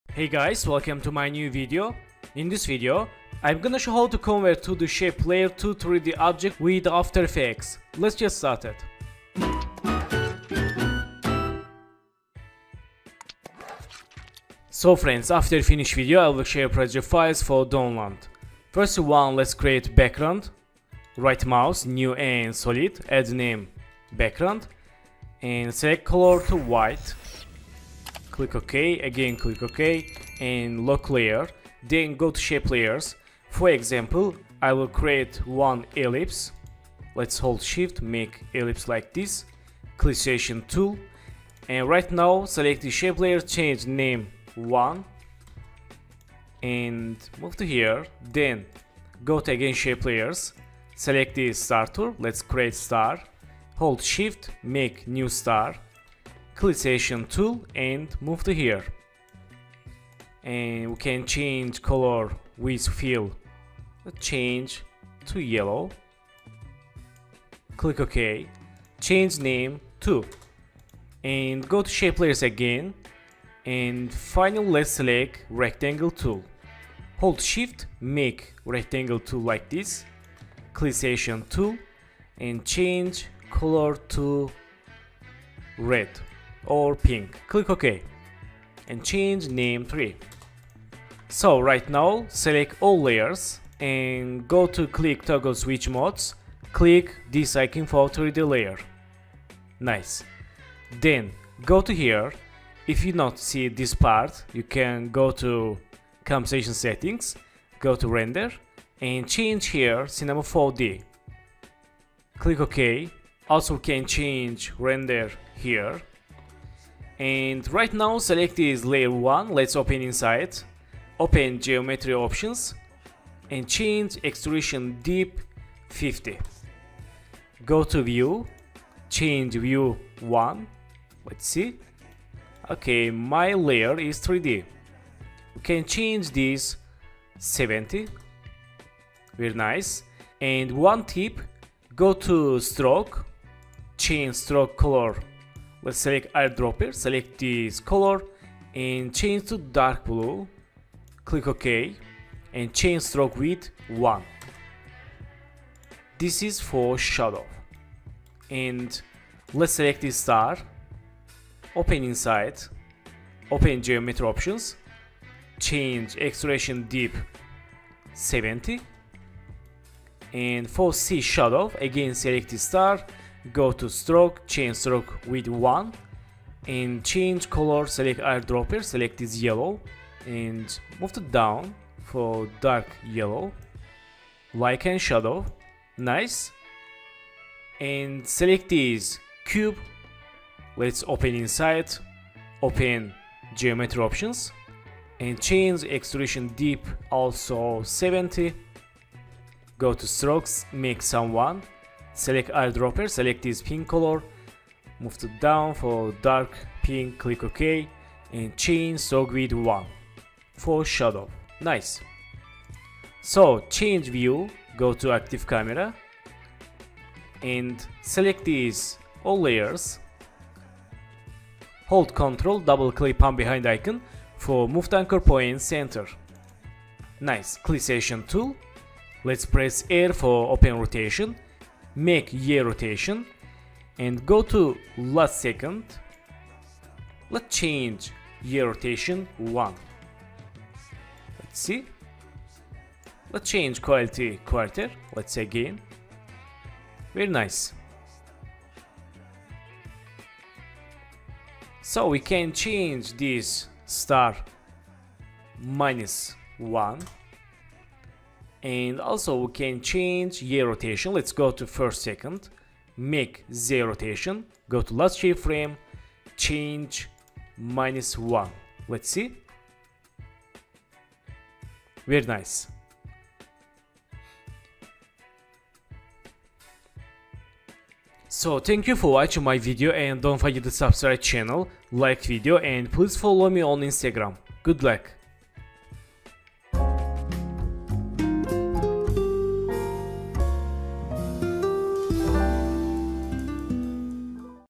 2D Shapes to 3D in After Effects | Basic Tips For Beginners
Hi guys, welcome to my new After Effects tutorial. In this tutorial i will show how to convert 2D shape to 3D object with After Effects. Let's get started!